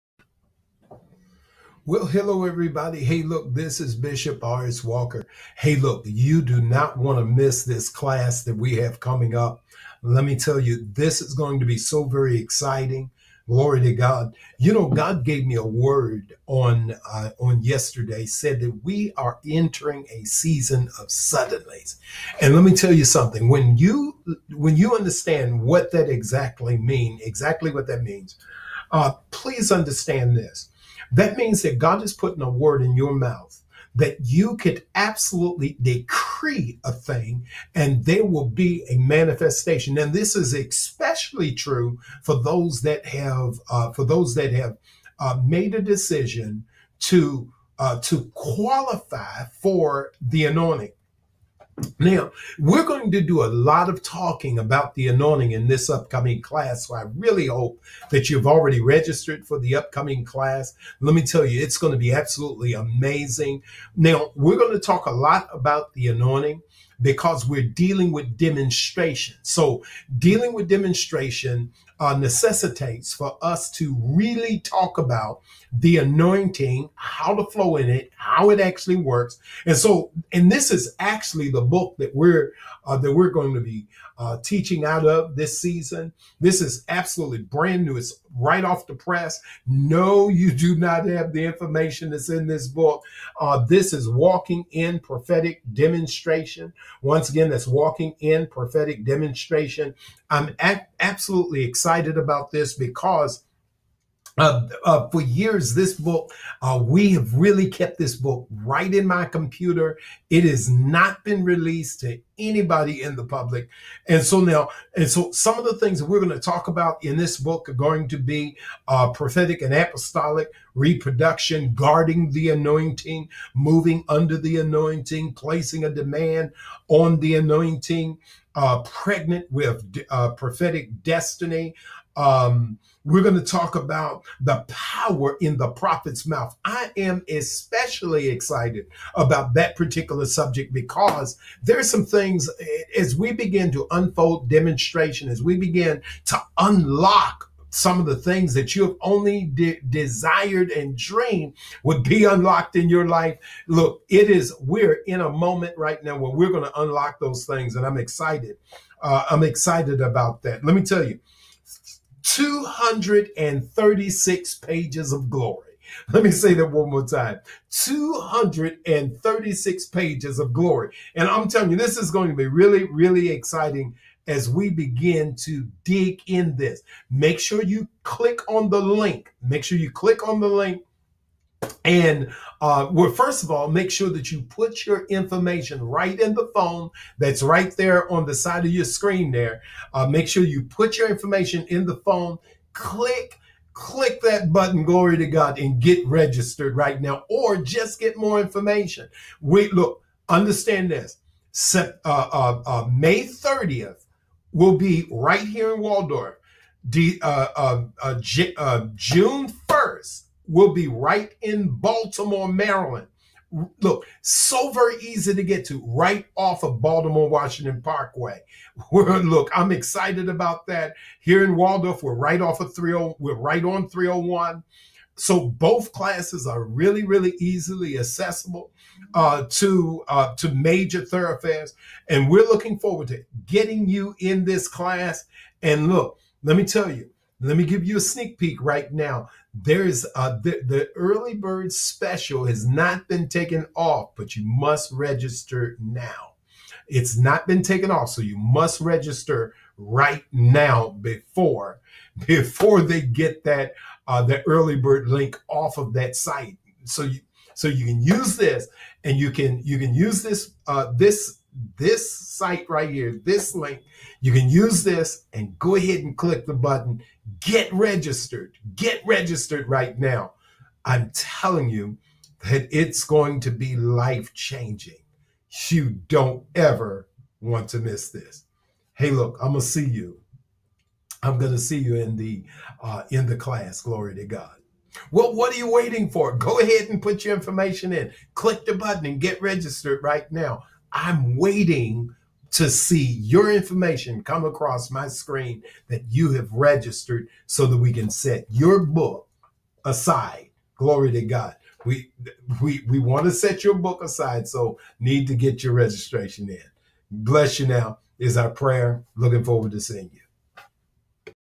School of Prophets two week Advertisement
Fresh new revelation on what God is doing in this season. We have been dealing with this emerging generation that God is speaking to and about. How do you train them? How do you handle them? What is God really saying about them? Get the answers here